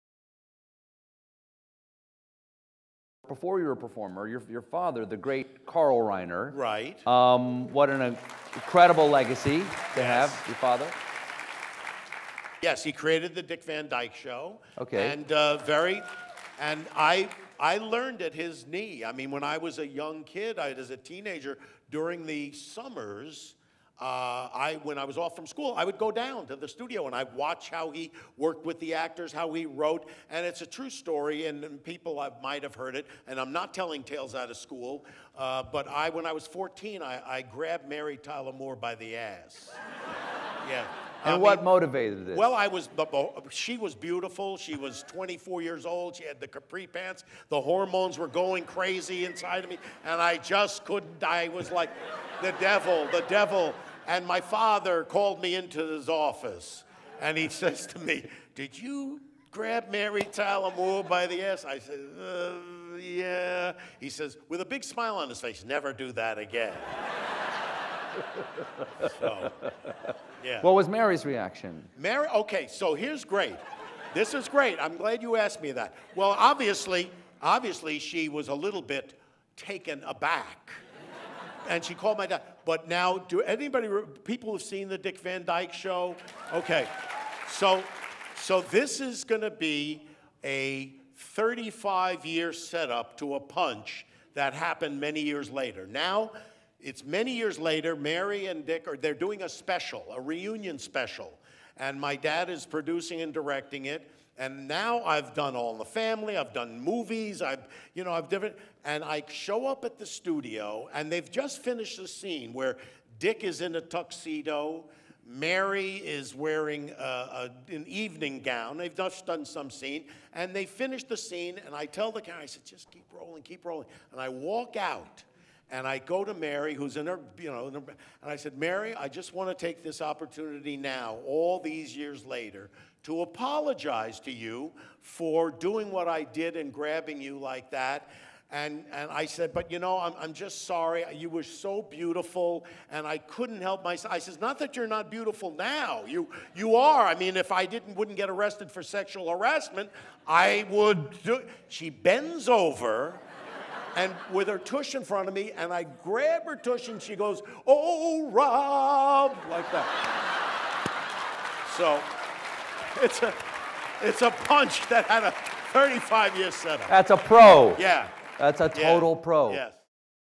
Rob Reiner Talks About Mary Tyler Moore
Rob Reiner reveals that when it comes to Mary Tyler Moore, he just can’t control himself.

Find out more information about the Montclair Film Festival on our